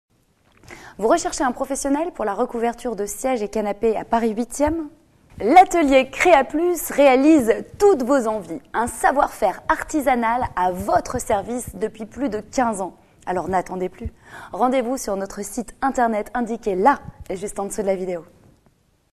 Recouverture de sièges et canapés Paris 8
Vous cherchez un tapissier pour la recouverture de sièges et canapés sur Paris 8ème ?
L’atelier CREAPLUS redonne vie à vos anciens meubles !
Tapissier décorateur sur Paris 8ème et la région parisienne, l’atelier CREAPLUS vous fait profiter d’un savoir-faire de plus de 15 ans.
Son équipe de tapissiers qualifiés prendront le plus grand soin de vos sièges et canapés : la garantie d’une recouverture parfaite, adaptée à vos gouts et envie !
De l’enlèvement de vos sièges et canapés à leur livraison à domicile sur Paris 8 une fois la recouverture réalisée, l’atelier CREAPLUS vous offre un service optimal !

"Aidez Sofian dans son combat contre l'ataxie telangiectasie"